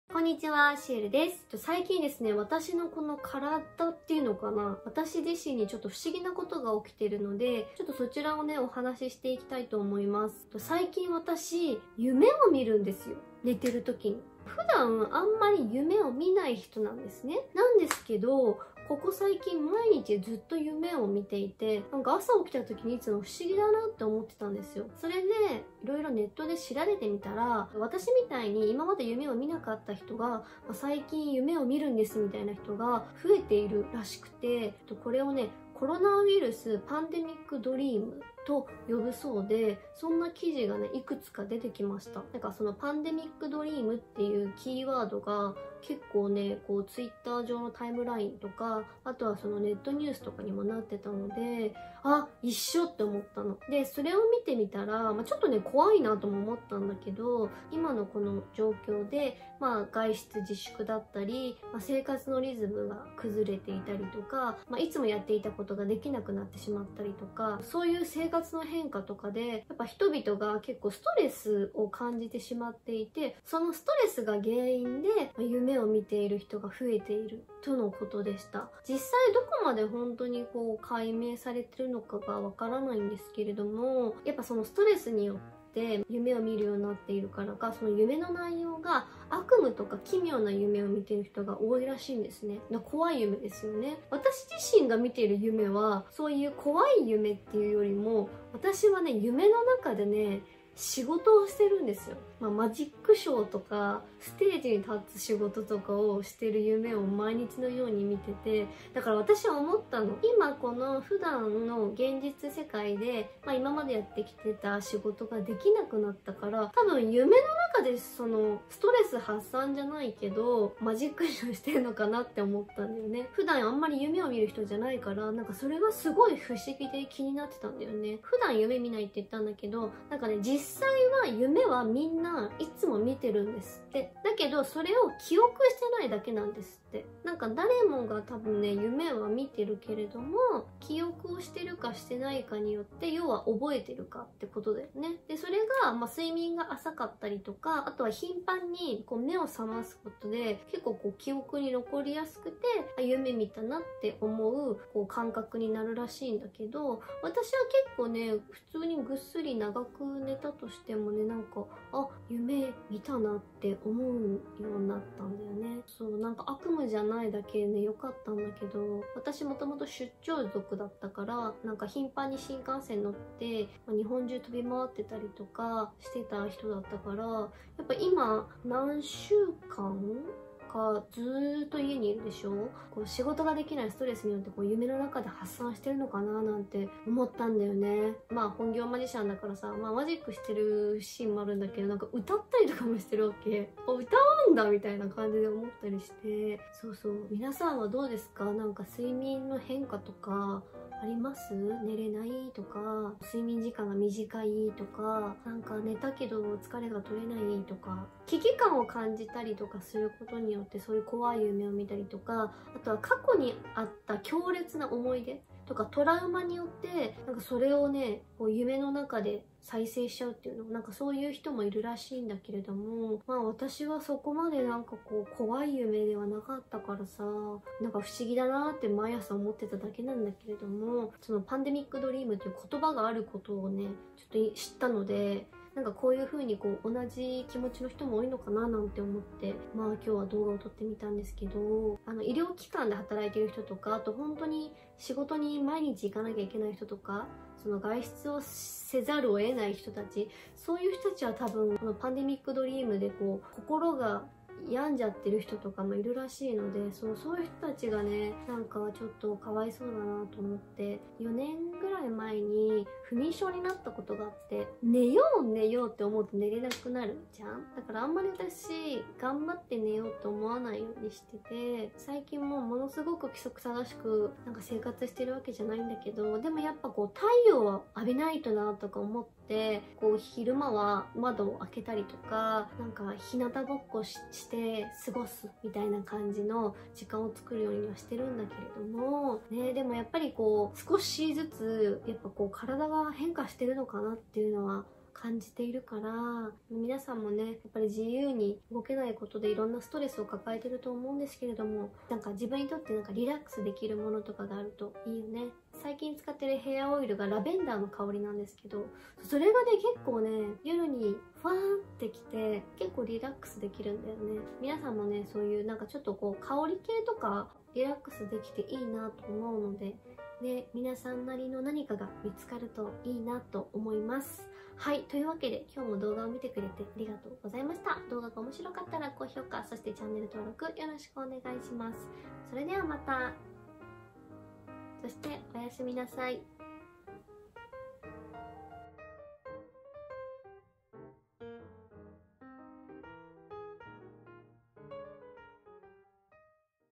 外出自粛のストレスが原因？一人暮らしの孤独？【パンデミック ドリーム】
【最近起きた体の変化】
毎日夢を見るようになった...その理由とは？
世界中で同じような体の変化を感じている人がいるようです。